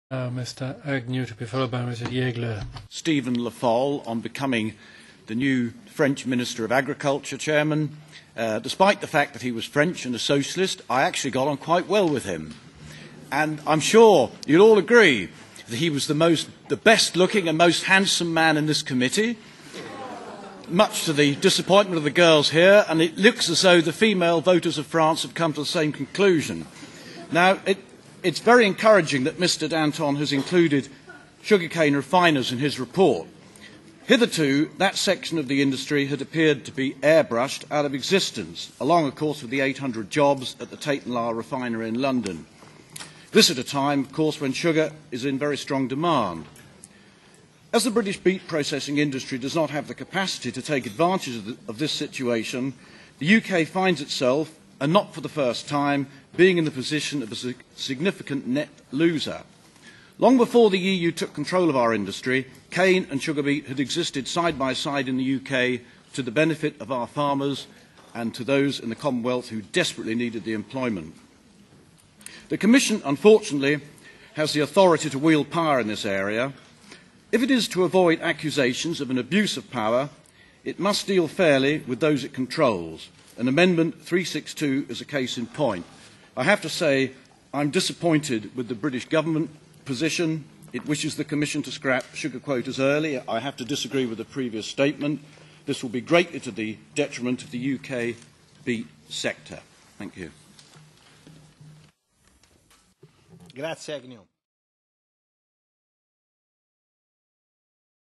UK beet sector abandoned by EU and HMG - Stuart Agnew MEP
• European Parliament, Brussels, 18 June 2012

• Speaker: Stuart Agnew MEP, UKIP (Eastern Counties), Europe of Freedom and Democracy (EFD) Group

• Item on Agenda: 4.0 Common organisation of the markets in agricultural products

- (AGRI/7/07522): Michel Dantin - Proposal for a regulation of the European Parliament and of the Council establishing a common organisation of the markets in agricultural products (Single CMO Regulation)